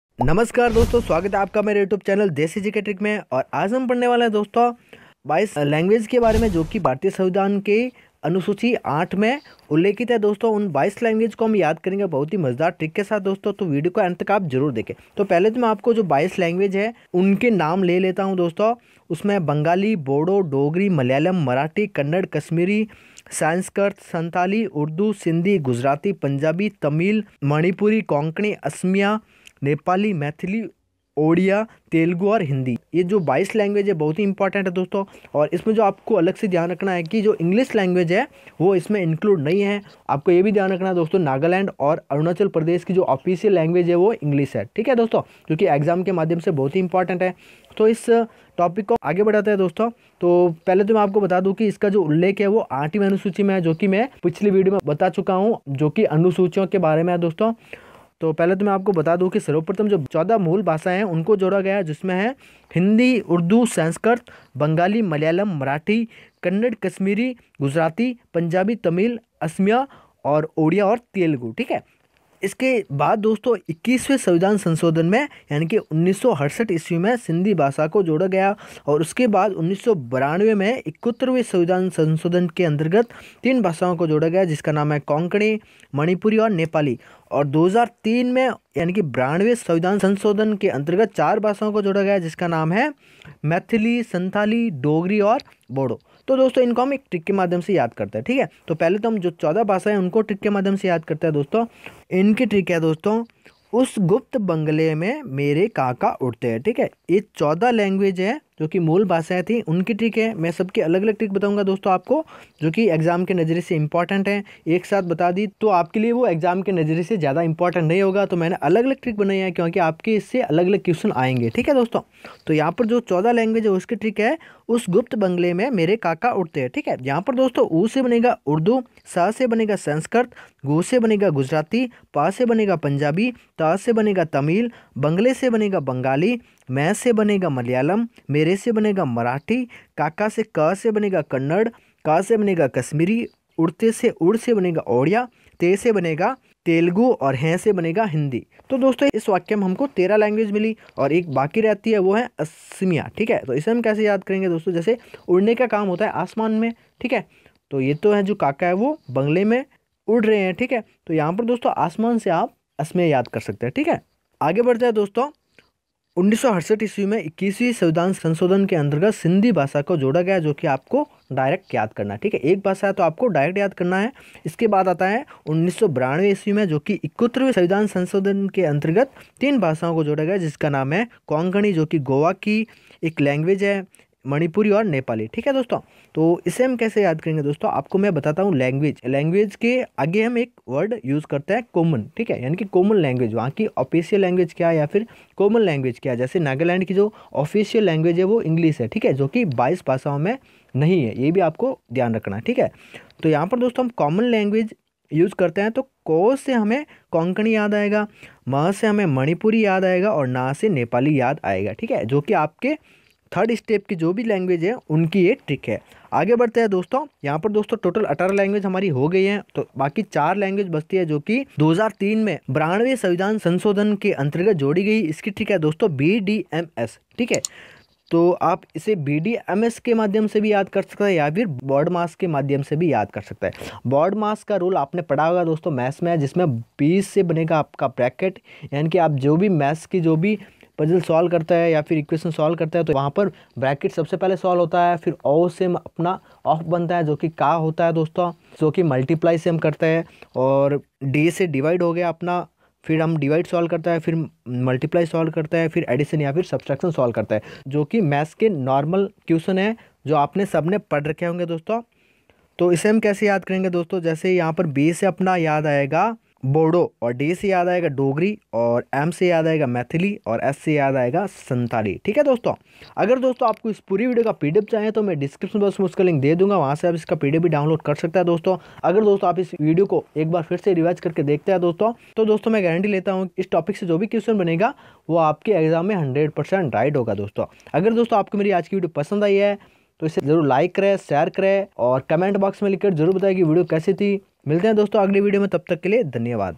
भारतीय संविधान की आठवीं अनुसूची में उल्लिखित 22 भाषा
भारतीय संविधान की आठवीं अनुसूची में उल्लिखित 22 भाषाओं  का एक शानदार ट्रिक के द्वारा वर्णन किया गया
SSC CGL,CHSL, CPO, MTS ETC EXAM
के लिए IMPORTANT QUESTIONS